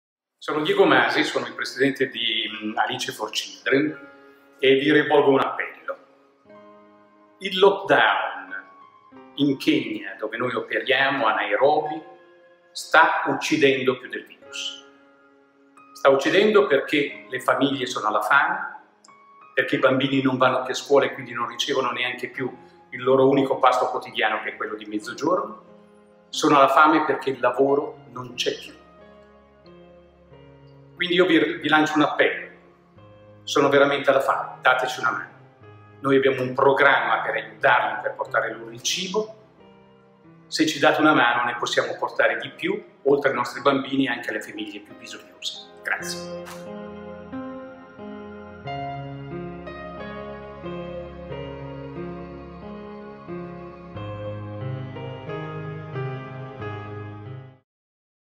LE FAMIGLIE DEGLI SLUM SONO ALLA FAME!
A Nairobi il lockdown ha messo in ginocchio l'intera comunità delle baraccopoli e le famiglie sono ridotte alla fame. Il nostro presidente Diego Masi chiede a tutti un aiuto perché la nostra associazione possa continuare a supportare uomini, donne e bambini degli slum
GRAZIE!